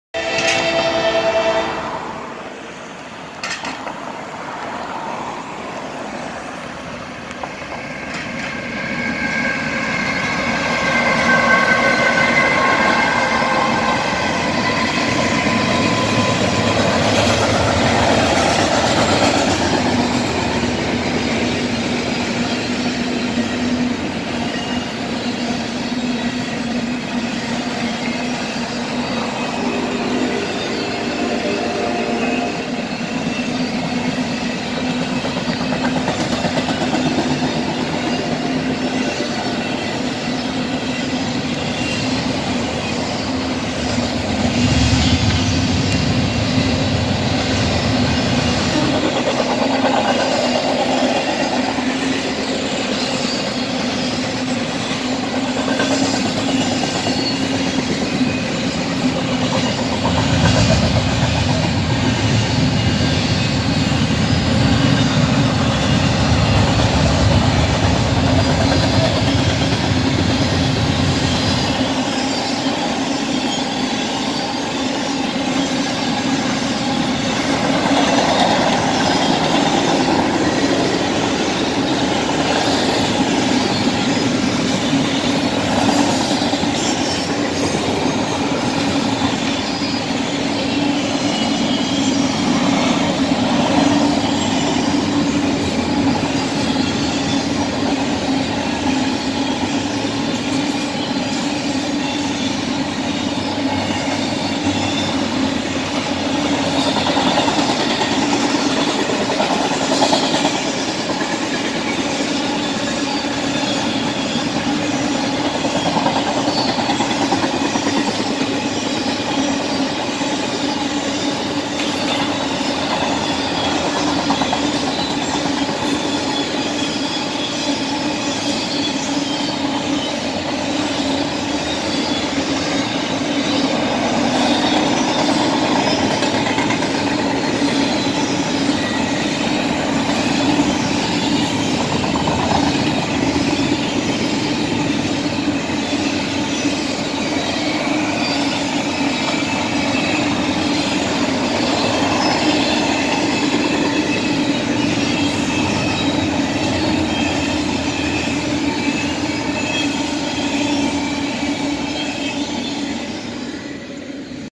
NS 221 in Suwanee, GA - 2/28/19
2 NS locomotives on February 28th's NS 221.